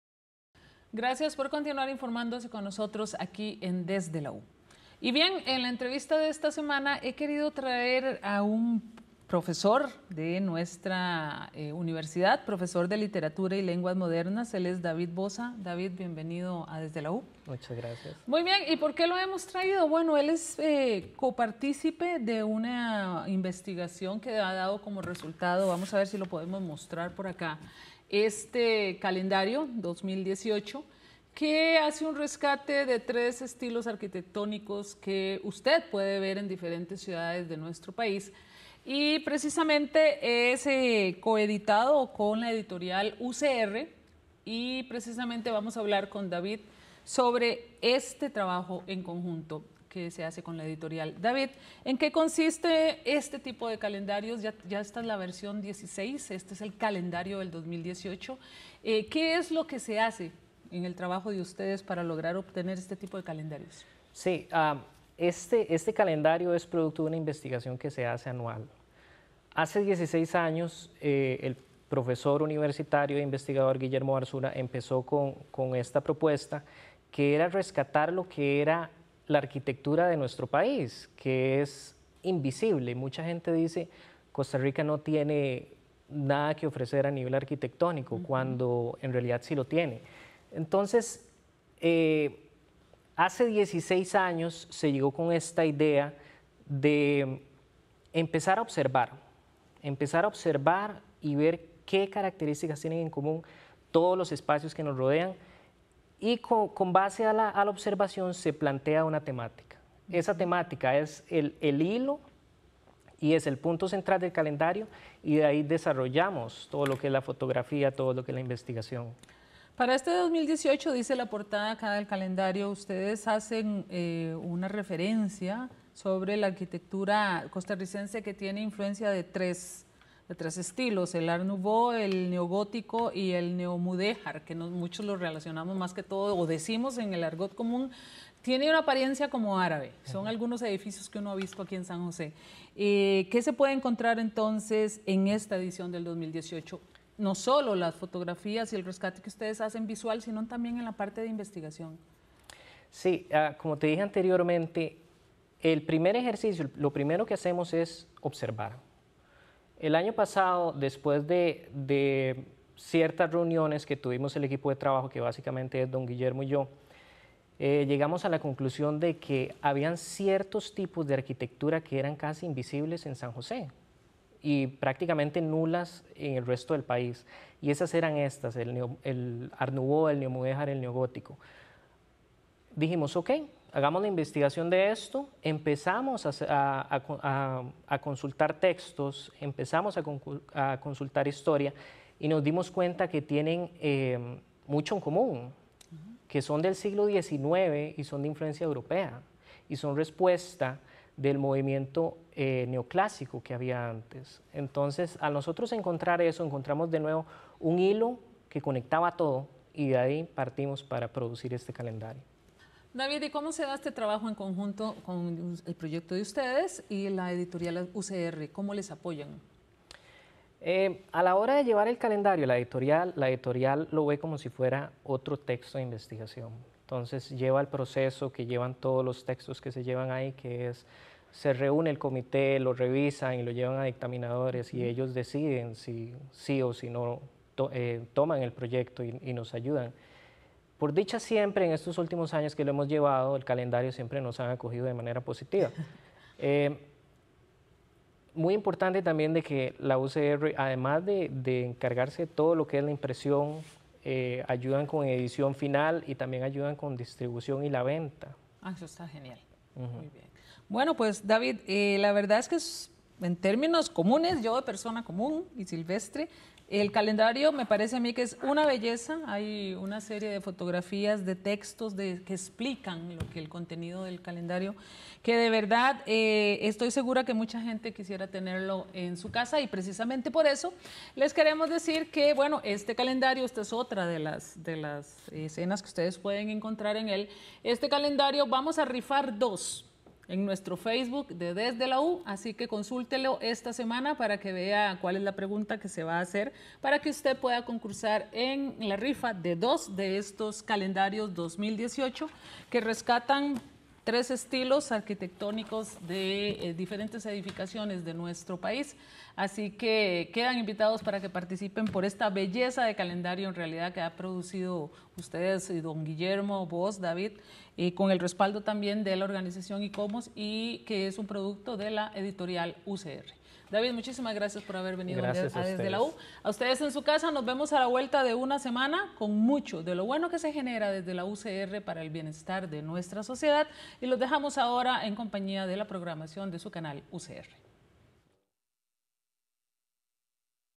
Calendario rescata la variedad arquitectónica de Costa Rica.
Calendario ICOMOS rescata la variedad arquitectónica de Costa Rica. Los detalles en esta entrevista con David Boza, docente de Literatura y lenguas modernas.